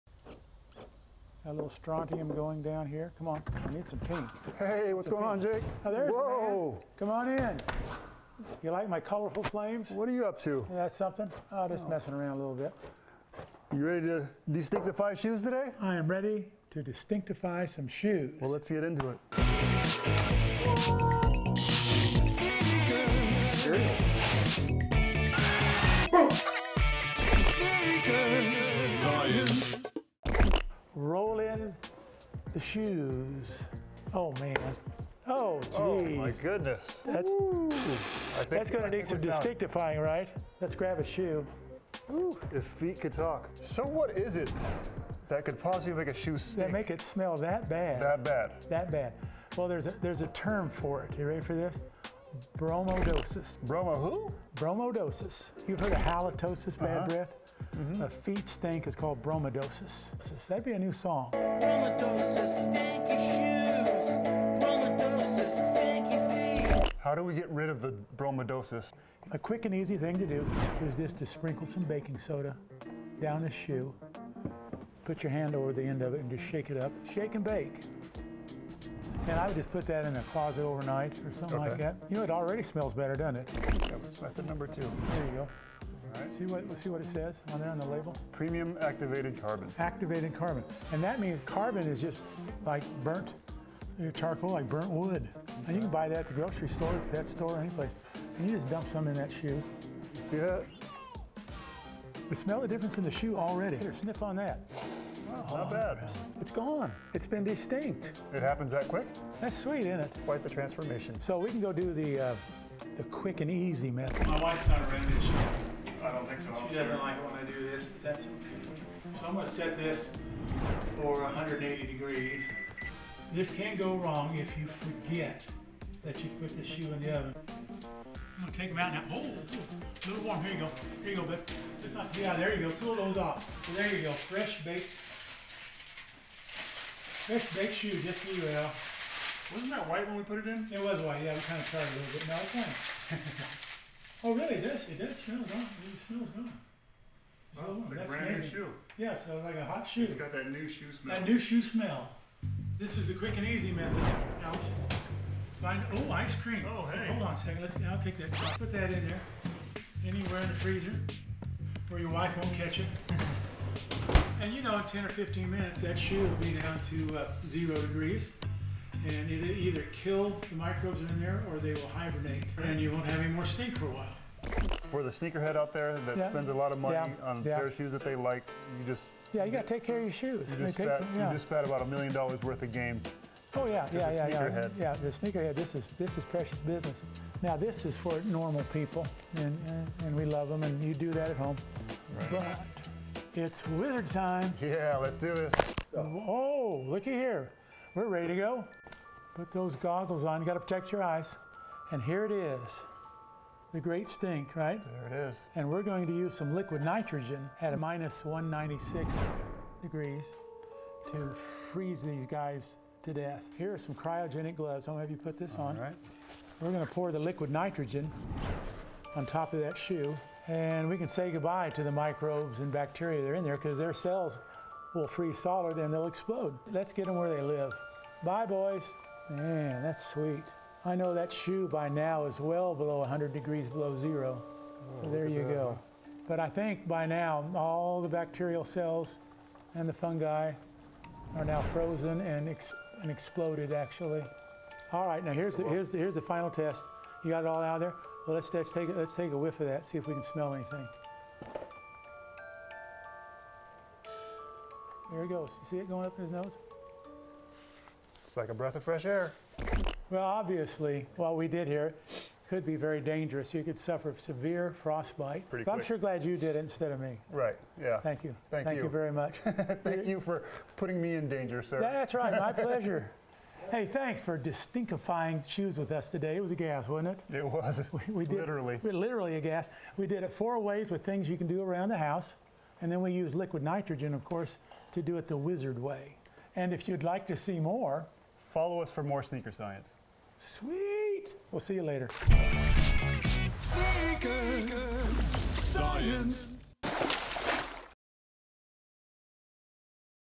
Destinkify Your Shoes // Sneaker Science Episode 1
In this episode of Sneaker Science, skater and shoe designer Alphonzo Rawls joins Jake (Wizard IV) to explore an issue plaguing sneakerheads everywhere... STINKY SHOES!

Want to see content like this early, get behind the scenes access, and be a part of future videos?

Ever discover that your favorite sneakers are just too smelly to wear? There's an actual scientific word for foot stench: bromodosis. But before you go throwing those sneakers in the washing machine or trash can, give science a chance! In this episode, we demonstrate four easy, around-the-house, DIY, science-to-the-rescue methods for "destinkifying" your sneakers... and then take things to the Wizard level.